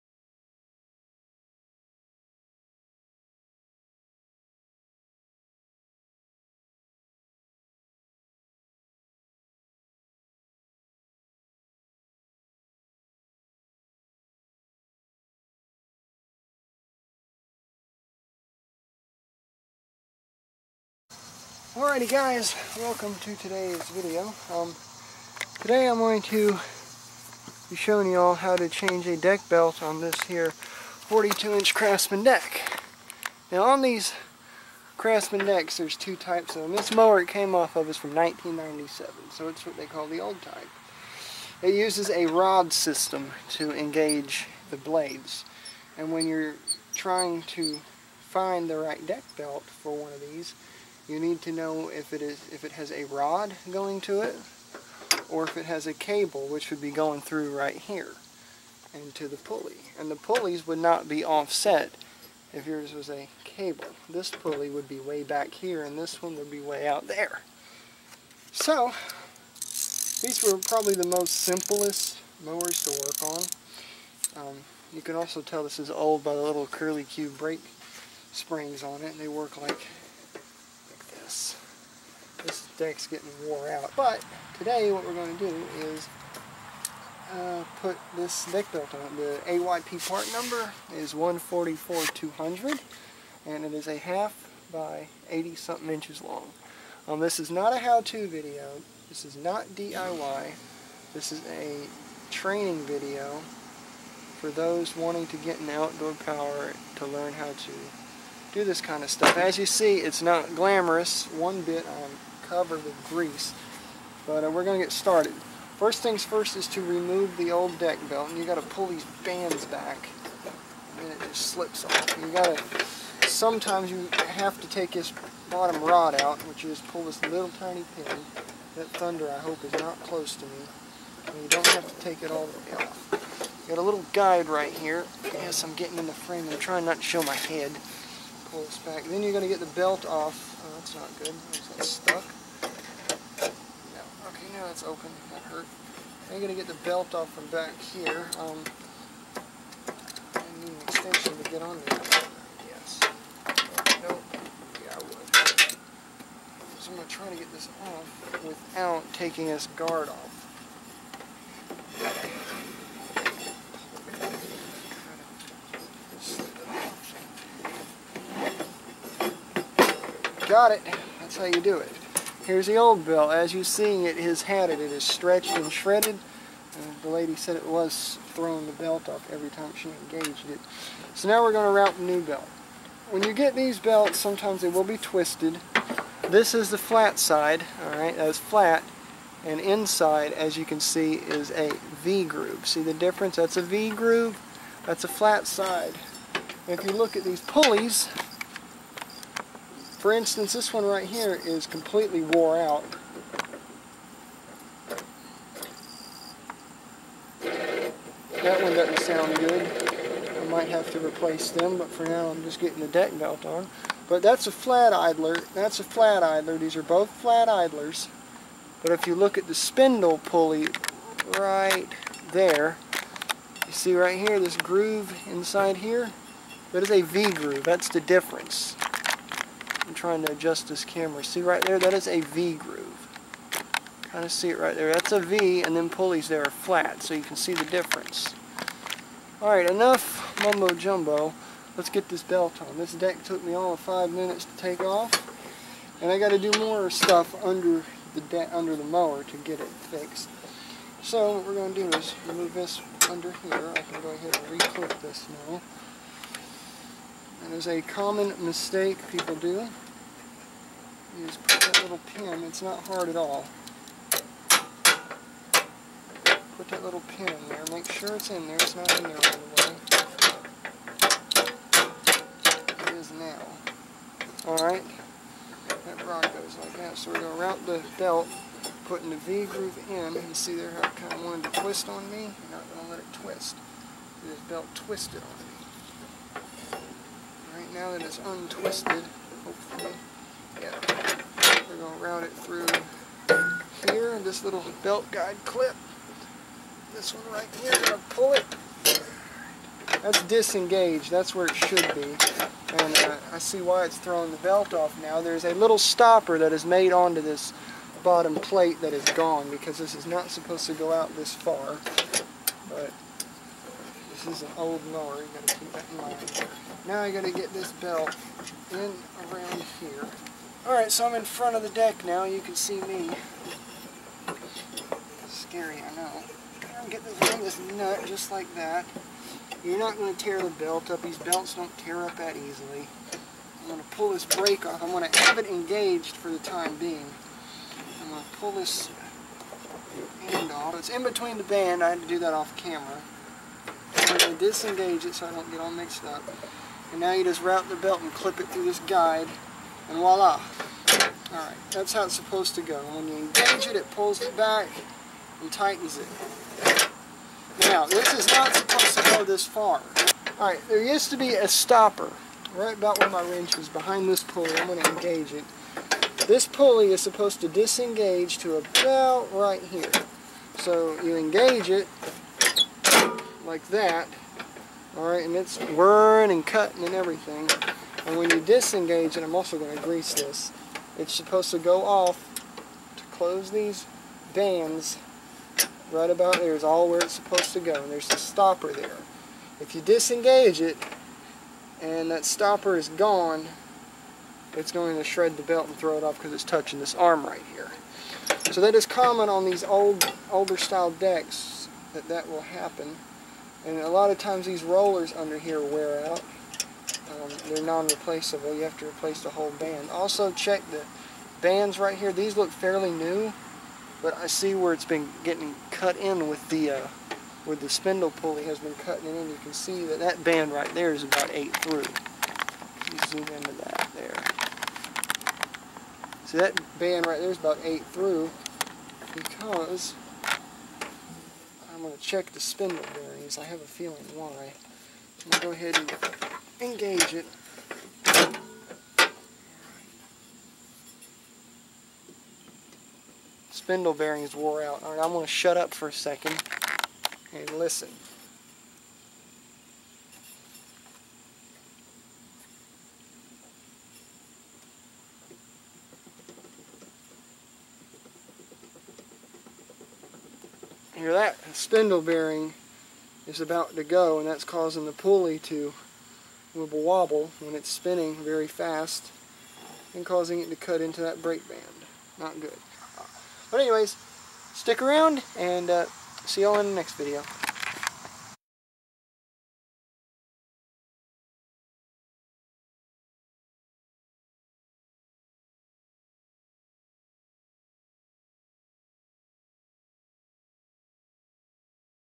Replacing The Deck Belt On An Old Style 42in Craftsman Mower
Originally Uploaded To TheOutdoorPowerGuy Channel on August,21 2016 Original Views 103,704.
In this video i show how to correctly replace a deck belt on a Craftsman/AYP 42" deck that uses the old style rod engagement system to engage the blades, this style was used from around 94 to the year 2000, after 2000, the engagement system was a cable type system that pulled the idler pulley and brake arm back to engage the blades, the cable system type uses a longer belt and the pulleys are not as off-set.  I will link a video via an annotation to a deck belt replacement i did on a cable type deck.